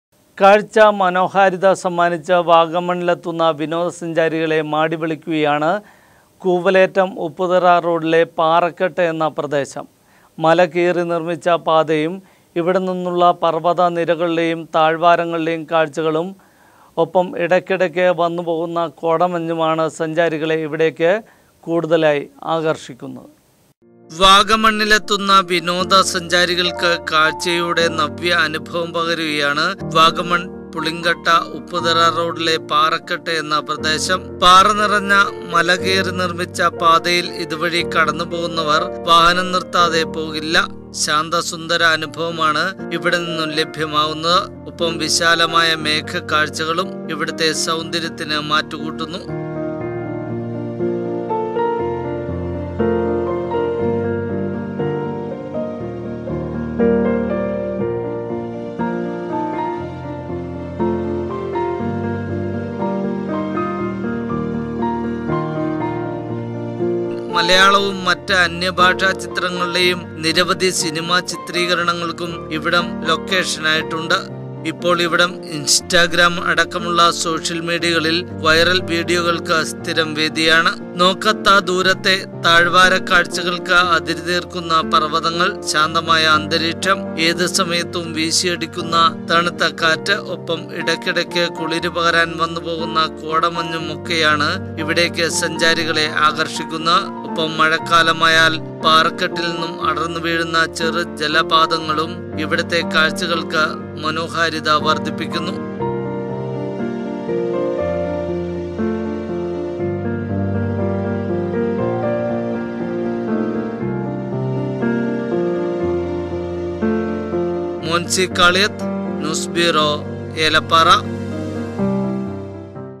വിനോദ സഞ്ചാരികളെ മാടിവിളിച്ച് കൂവലേറ്റം ഉപ്പുതറ റോഡിലെ പാറക്കെട്ട്
കാഴ്ചാ മനോഹാരിത സമ്മാനിച്ച് വാഗമണ്ണിലെത്തുന്ന വിനോദ സഞ്ചാരികളെ മാടിവിളിക്കുകയാണ് കൂവലേറ്റം ഉപ്പുതറ റോഡിലെ പാറക്കെട്ട് എന്ന പ്രദേശം. മല കീറി നിര്‍മ്മിച്ച പാതയും, ഇവിടെ നിന്നുള്ള പര്‍വ്വത നിരകളുടെയും താഴ് വാരങ്ങളുടെയും കാഴ്ചകളും,ഒപ്പം ഇടയ്ക്കിടയ്ക്ക് വന്നു പോകുന്ന  കോട മഞ്ഞുമാണ് സഞ്ചാരികളെ ഇവിടേക്ക് ആകര്‍ഷിക്കുന്നത്.